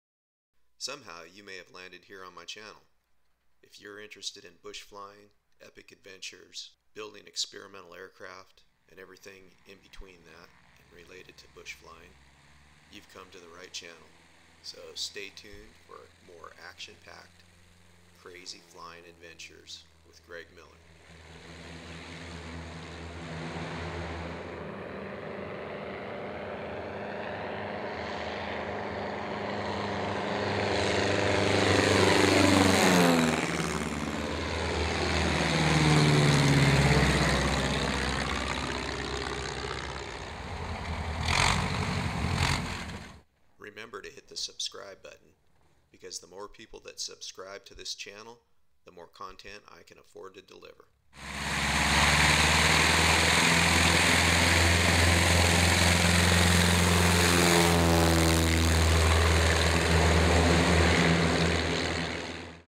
Trailer for channel
If you have landed on my channel you are probably interested in flying airplanes in some way.   My name is Greg Miller and I live in the Pacific Northwest of the United States of America.  The world of off airport flying sometimes referred to as Bush Flying is a growing interest and I have been around since the start of it going mainstream.

I started making flying DVD's over 15 years ago of "Off Airport Flying".  The series Big Rocks and Long Props Volume 1-5 was some of the first look into this unique form of off runway or off the beaten path of extreme flying.  Since that time, many others have taken up this new way of using an airplane.

I use my airplane for adventure.  The skills I have honed over a period of 25 years have taken me on many adventures that most only dream about.  If you like the outdoors like I do, then follow along on some old and some new adventures.  We will explore hunting, fishing, dirt bike riding, white water rafting, to name just a few.  I will continue to build content and bring more of what you are asking for in the comments section.

I am posting this in response to a few negative posts I have seen.  By the way, my channel, my rules.   I will remove them when I see them so you might as well not waste your time.  Give me a thumbs down if you don't like this video, or any of the other videos and leave my channel.  That is the best way to vote here.

Sincerely,

Greg Miller